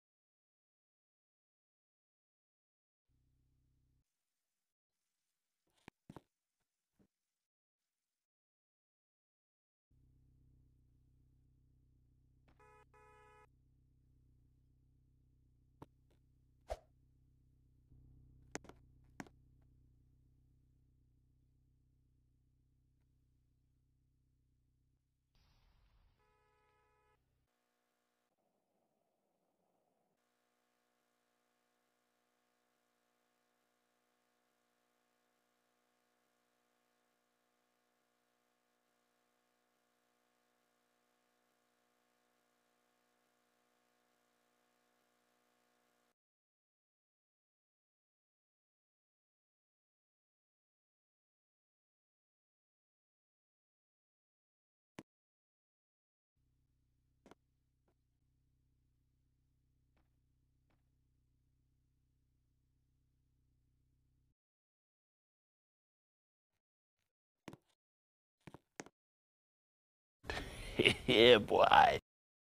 When a customer says werid names
In today's video this is where customer says werid names and yeah you are just really confused comment down if you have ever met someone with werid names hope you liked and subscribed the video enjoy!